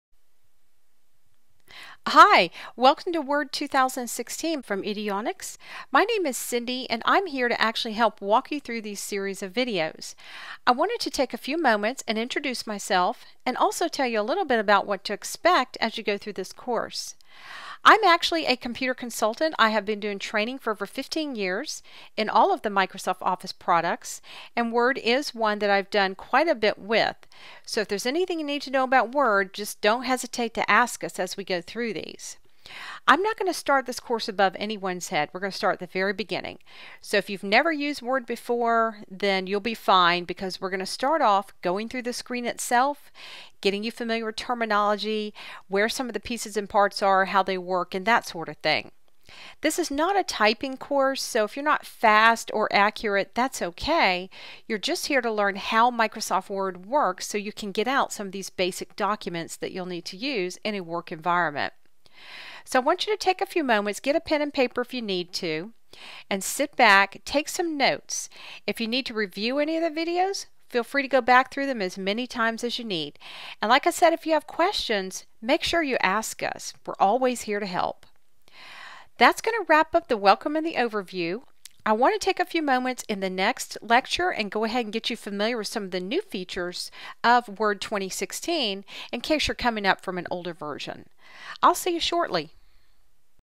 Quick and Easy Guide to Microsoft Word - Intro
Enhance your skills on the most used text editor. Learn simple & complex features of Word 2016 in this Word tutorials